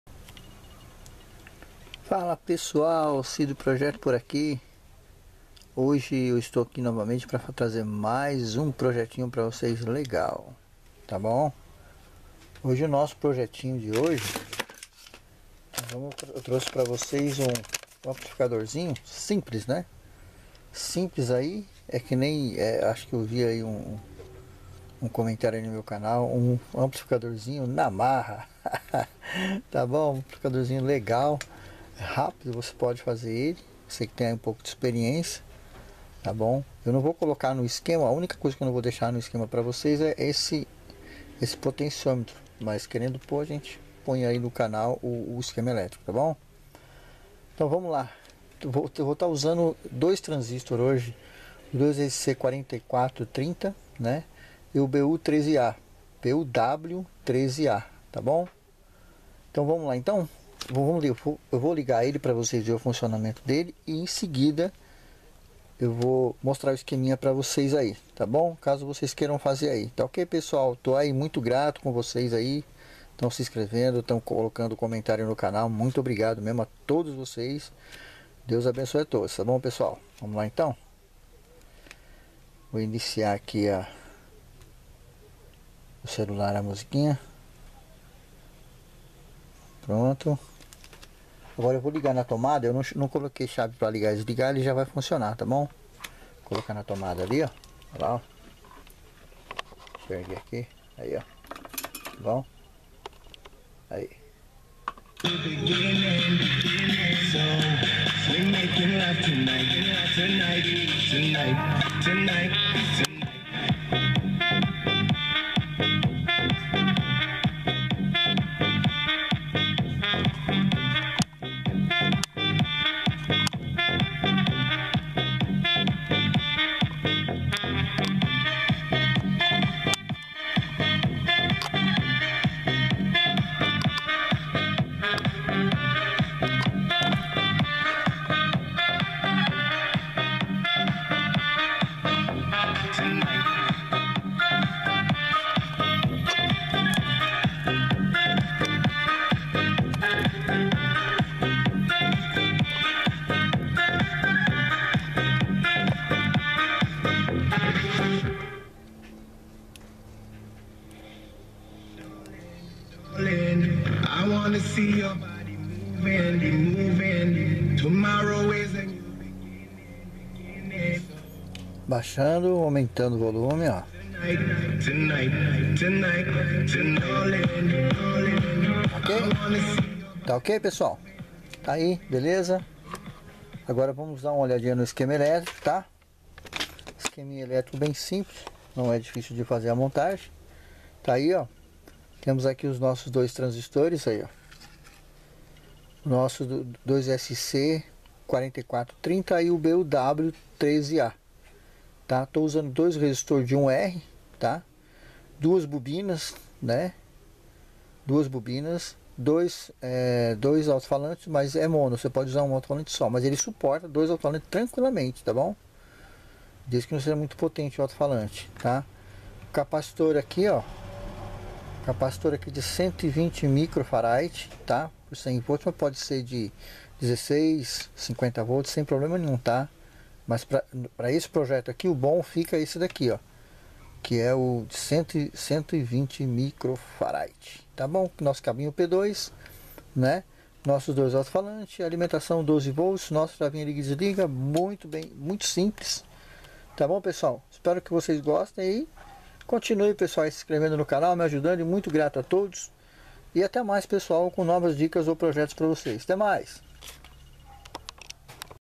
amplificador caseiro com transistor 2sc4430 e buw13a
tai tura espero que voces gostem um amplificador caseiro muito facil espero
que eu possa ter agradado tabom um grande brigadoooooooo a todos